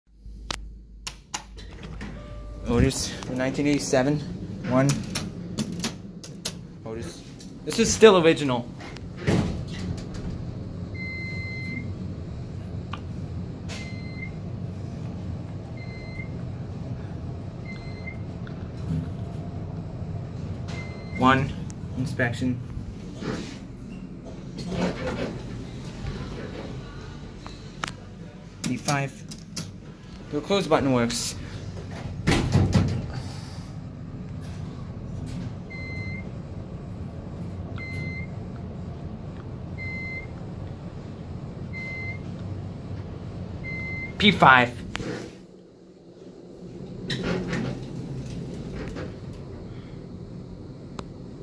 31 August 2019: 1987 Otis BD Traction Elevator 15 | Bellevue Place | Bellevue WA
Another take from my backlog.

Taken 31 August 2019

Installed 1987